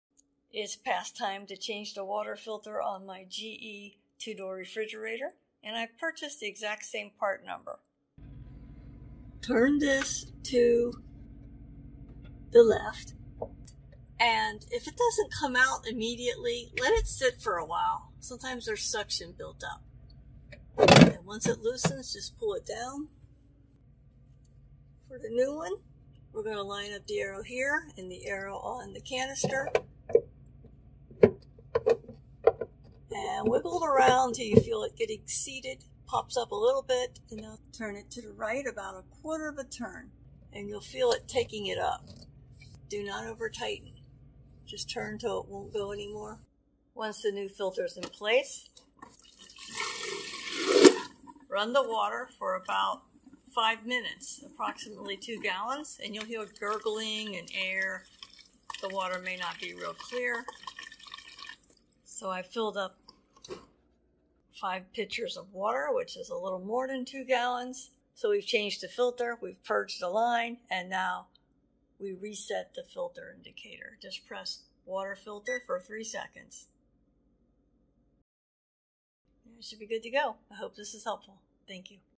GE Side by Side Refrigerator - Change Water Filter - XWFE
Change filter, purge water line, and reset filter indicator - Quick & Easy!!

Two door french door GE refrigerators.